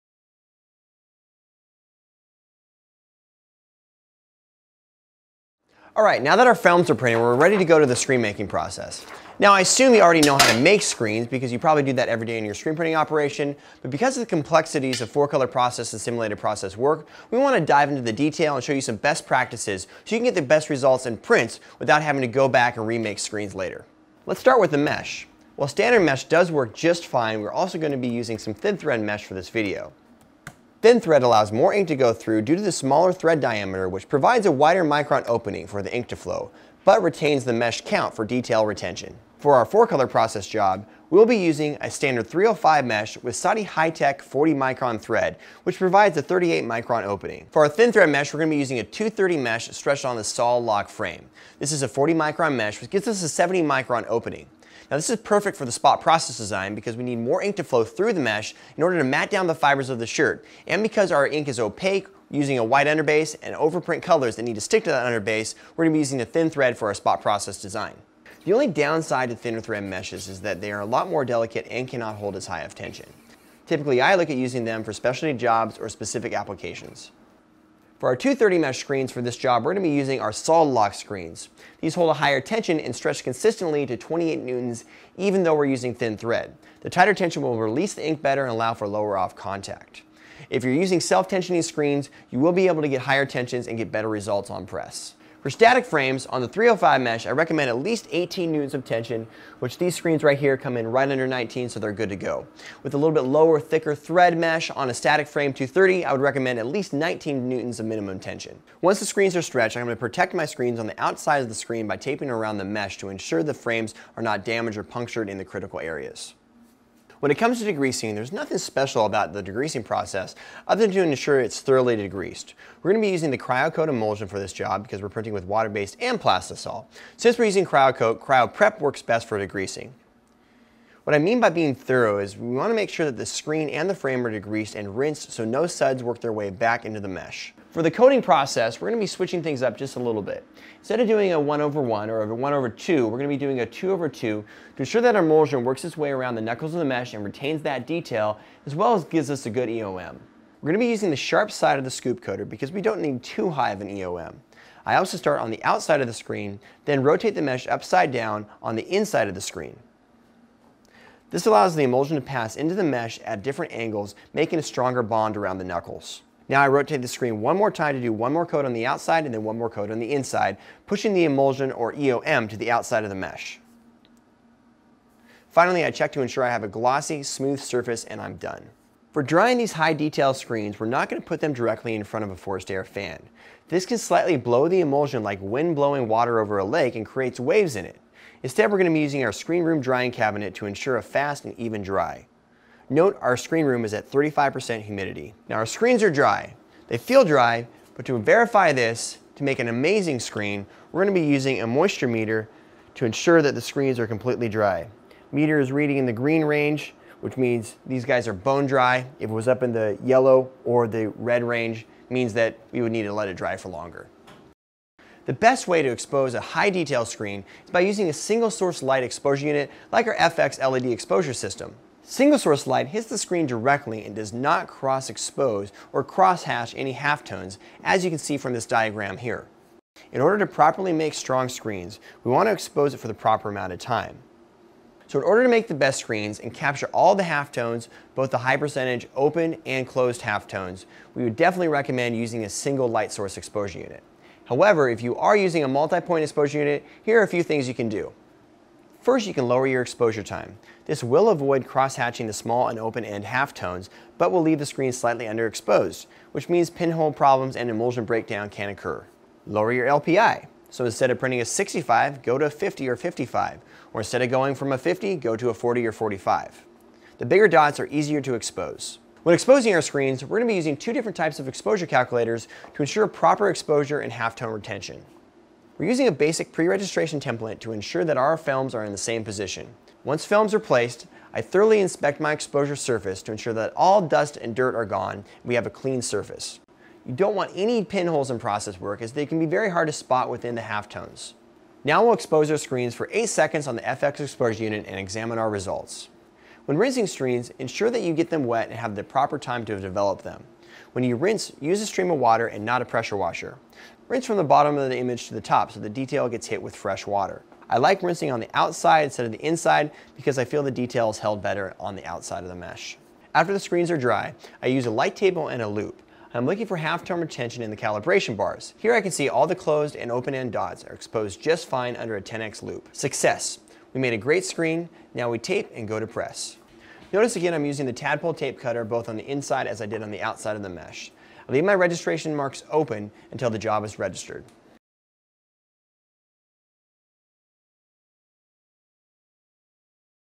How to Prep and Coat Screens for Process Screen Printing
Ryan goes over the best practices for creating screens for four and simulated color processes.